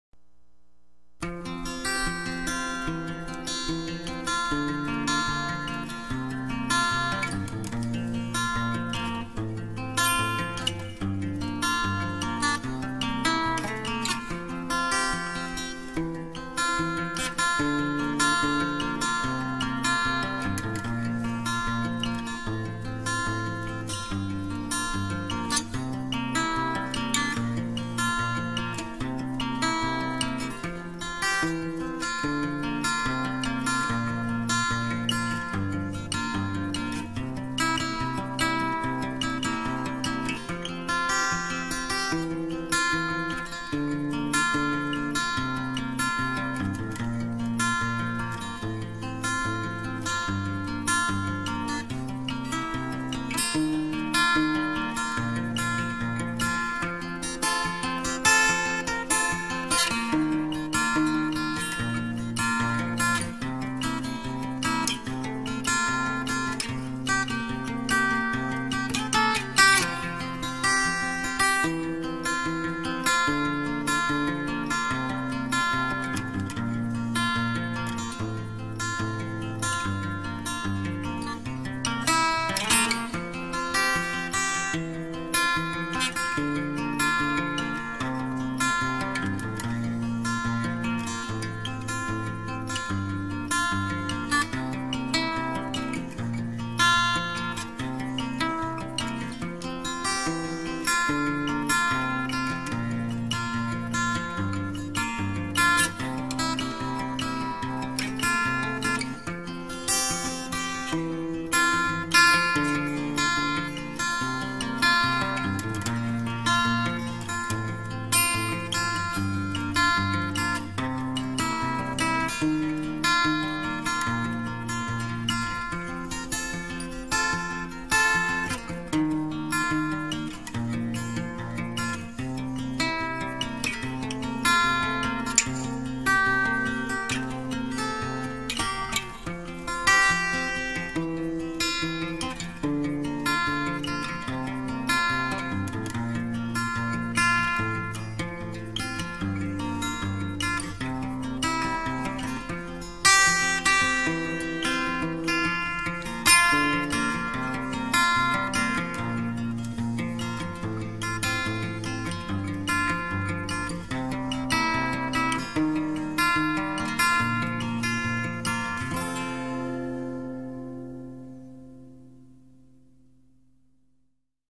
Watercolour Painting Bees and Honeycomb Timelapse
Timelapse video of two bees and honeycomb watercolour and ink pen painting. Honeycomb painting timelapse tutorial.